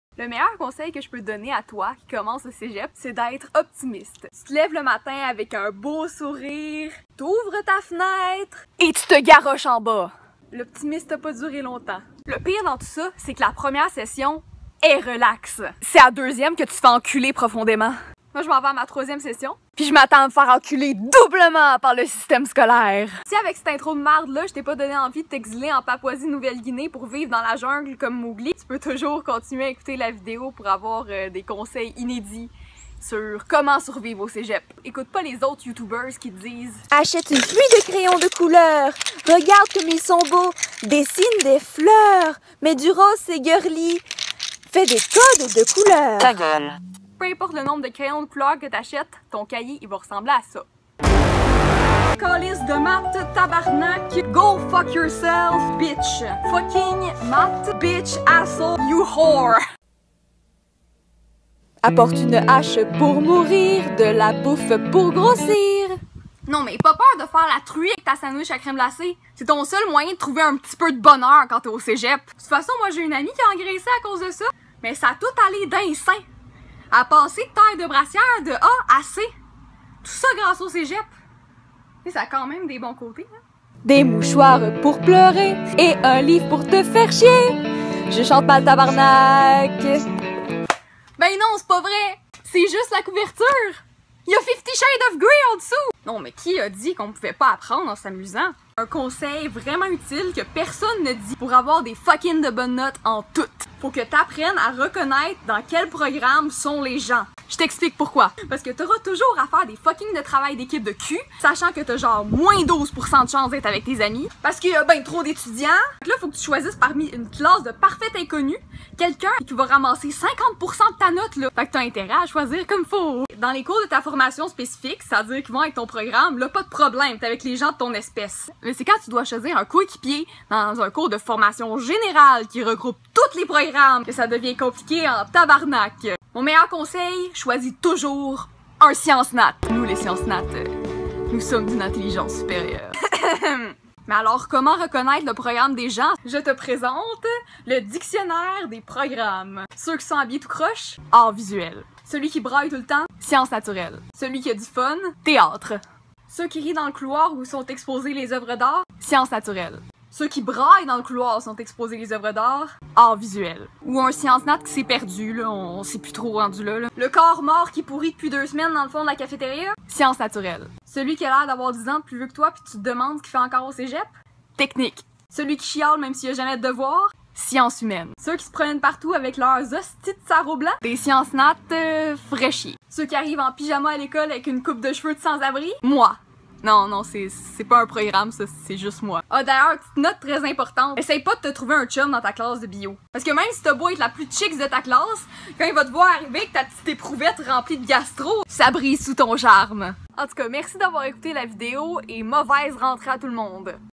MES MEILLEURS CONSEILS POUR LA RENTRÉE AU CÉGEP
Désolé si c'est un peu très vulgaire, je ne peux pas être heureuse en parlant de l'école ... x)